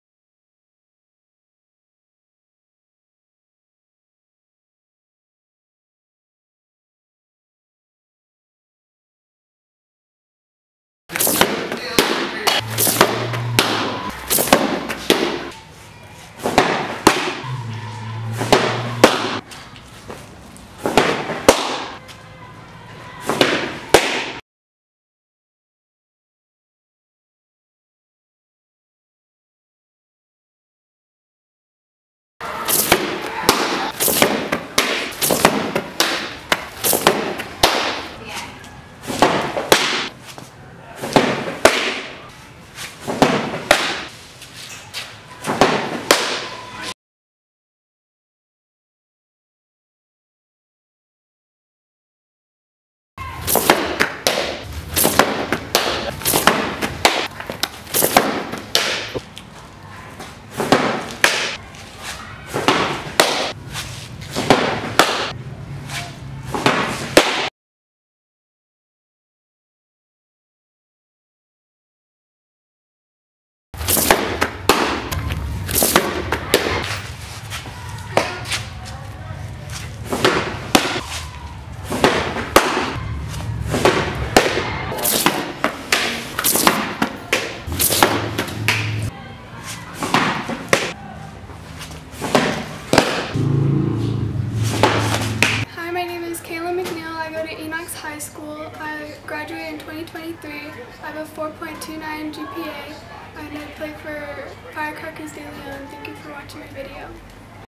Kaylin McNeill 2023 fast pitch pitching college recruiting video.
Fast pitch pitching video with 2023 college recruit Kaylin McNeill , Pocket Radar speeds and Rapsodo movement included.